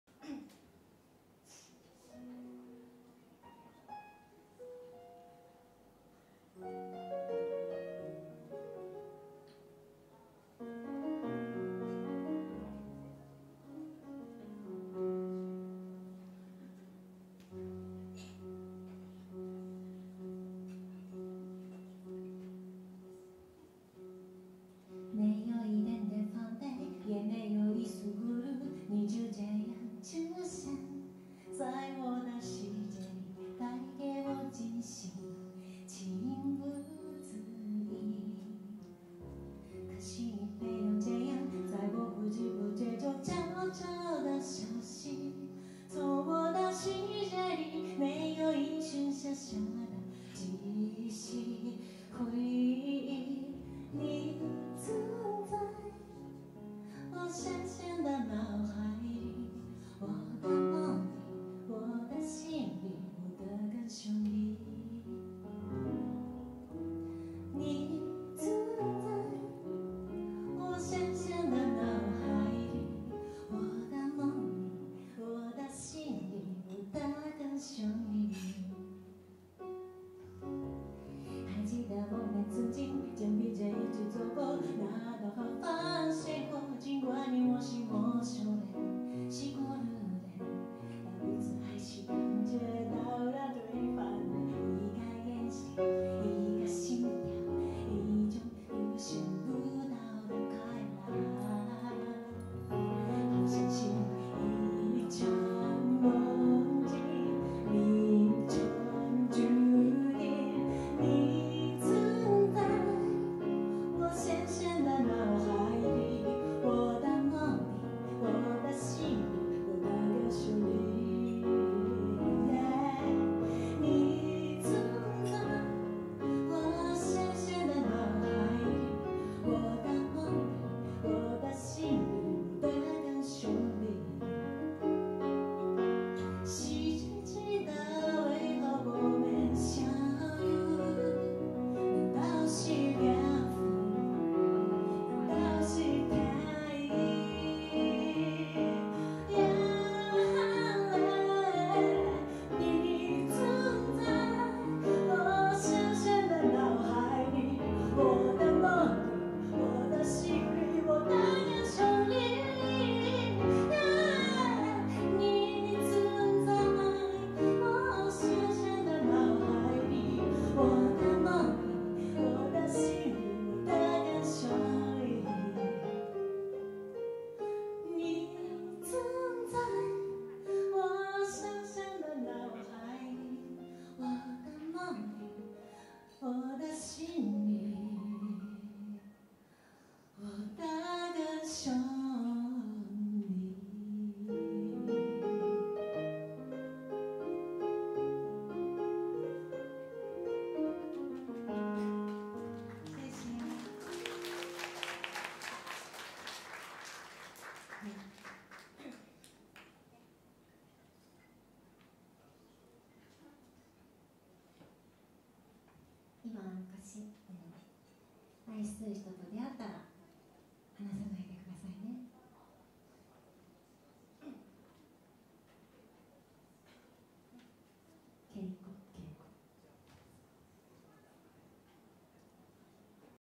MakiLive20130824M08
2013年8月24日、上海のライブカフェ『开心果』(KaiXinGuo)本店にて行われた、Makiさんのライブの模様です。

Vo：行川麻紀子
Pf：田上敬子

撮影＆PA：林陽一